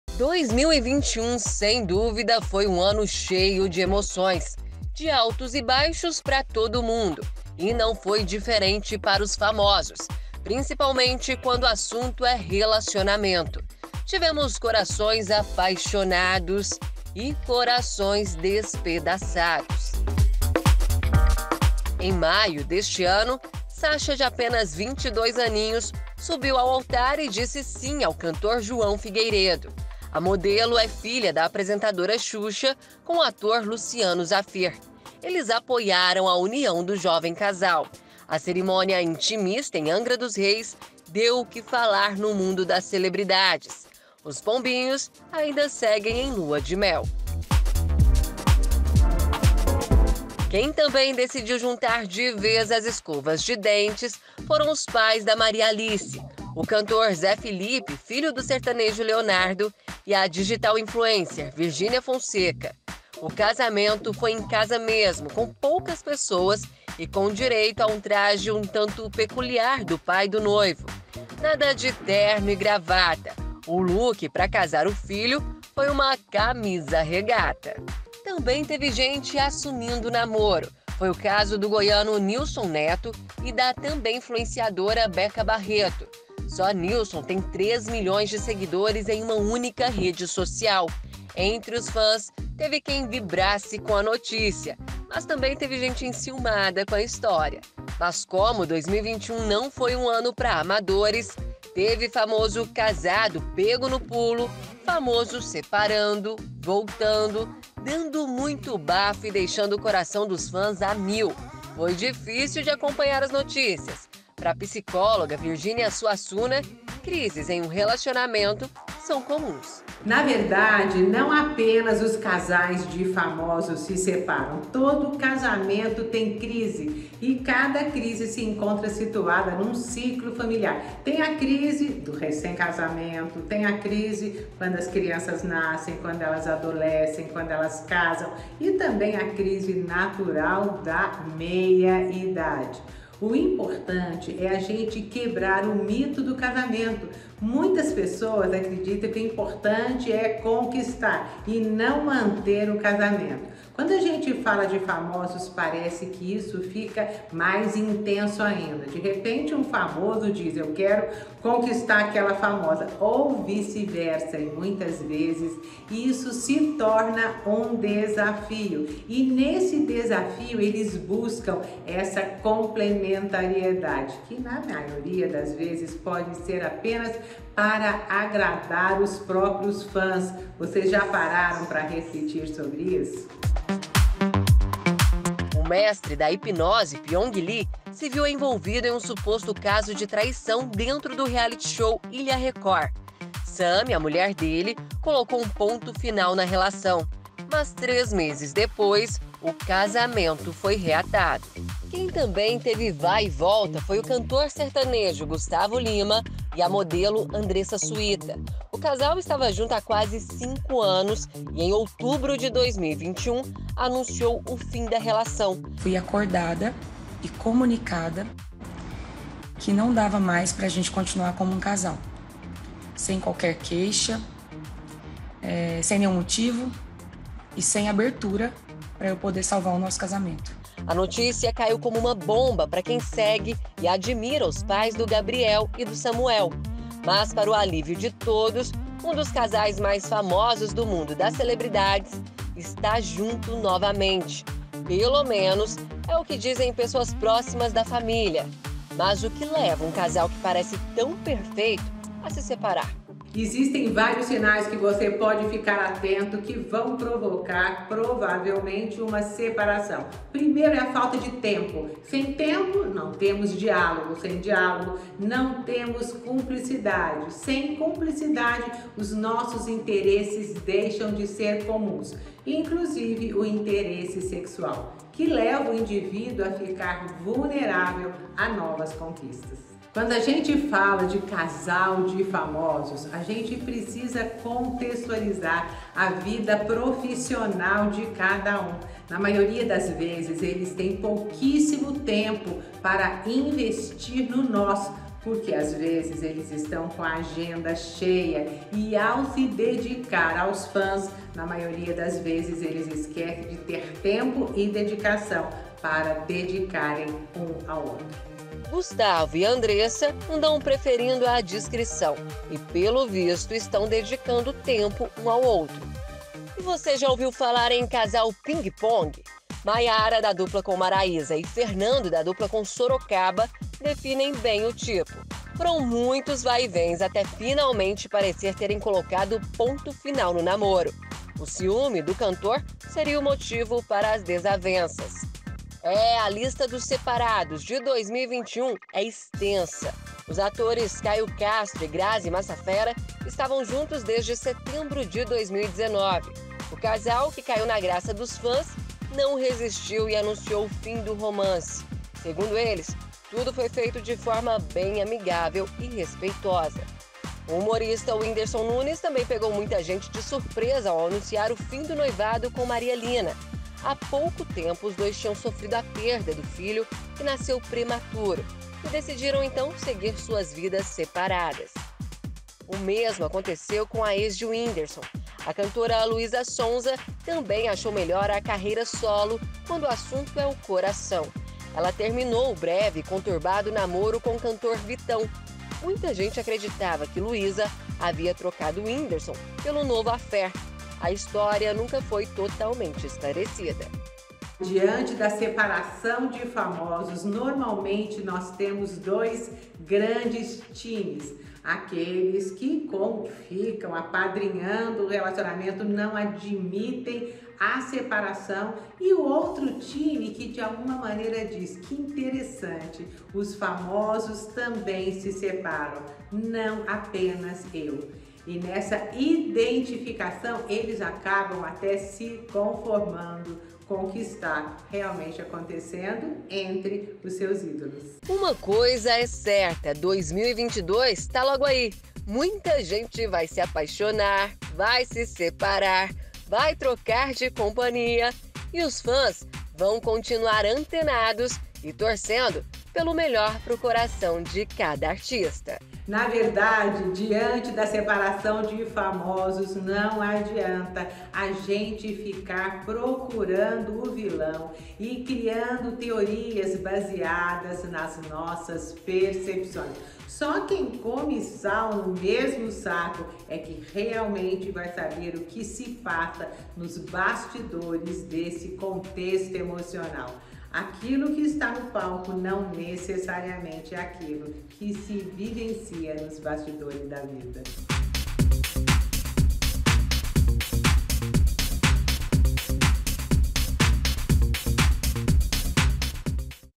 IDAS E VINDAS OS RELACIONAMENTO DOS FAMOSOS EM 2021
O ANO DE 2021 FOI MARCADO POR MUITOS ROMPIMENTOS ENTRE CASAIS FAMOSOS. MAS POR OUTRO LADO TEVE MUITA GENTE UNIDO AS ESCOVAS DE DENTES.